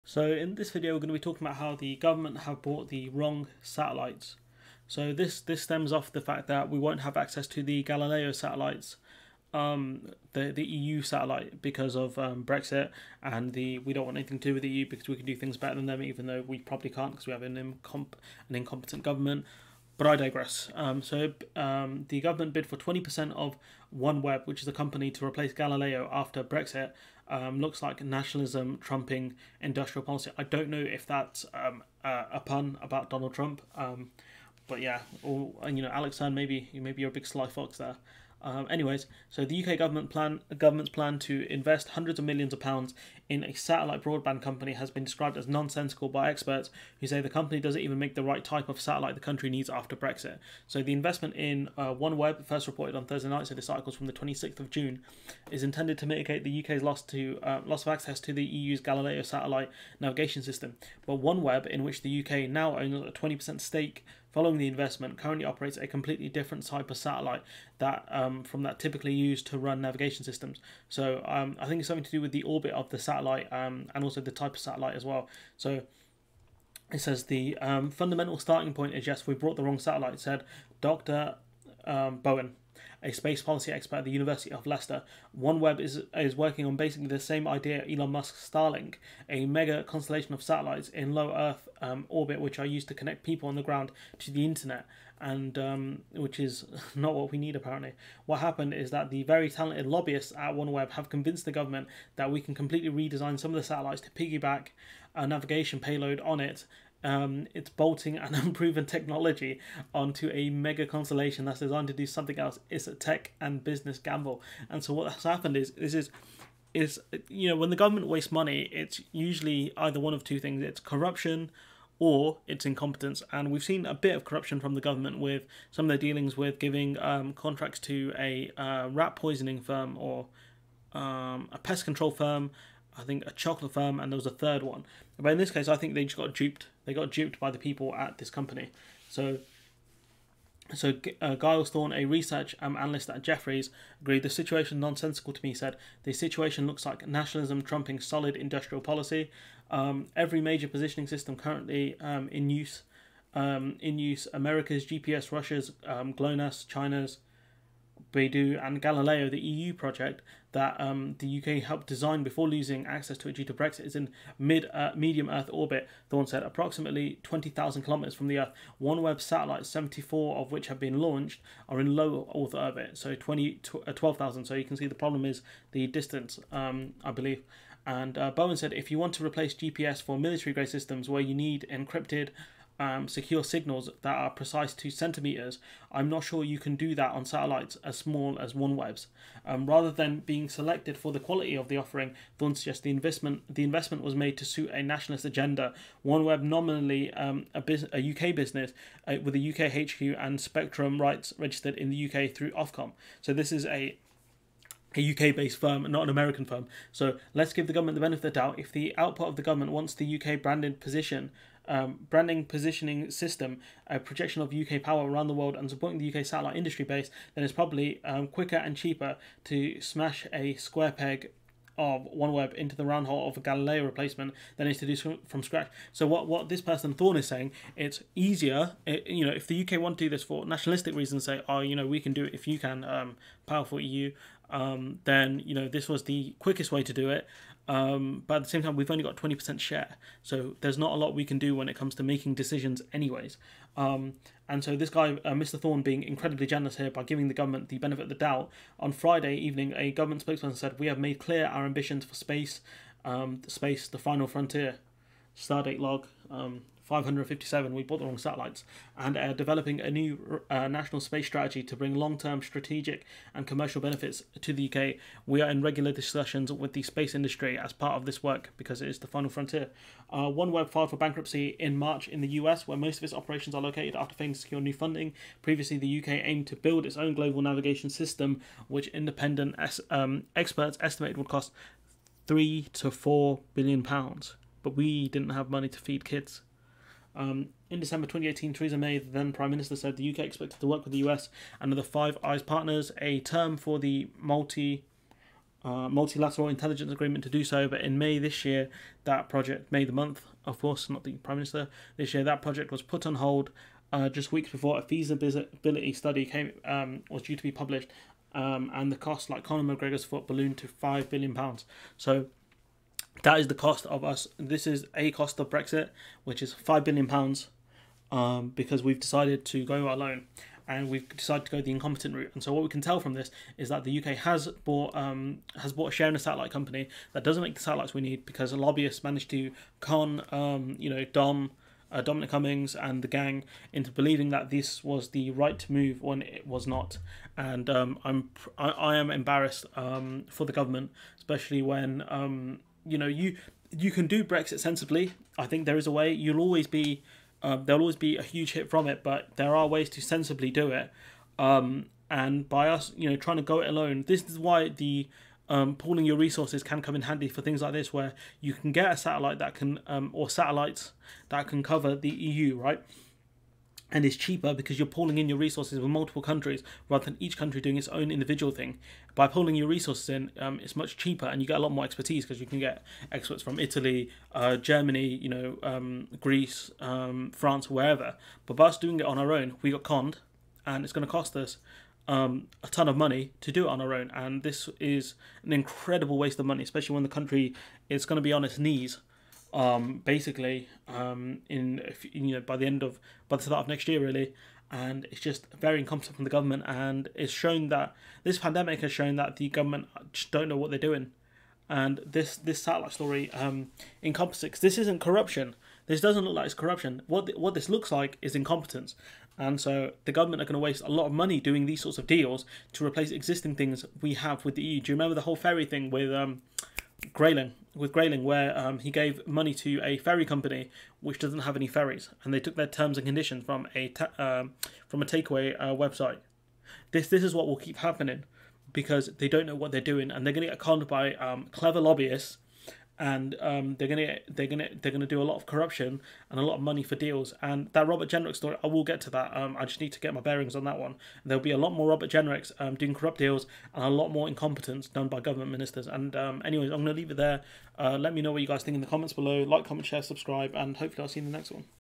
UK government brought the wrong satellites, invested in a failing company
Shock
Reference: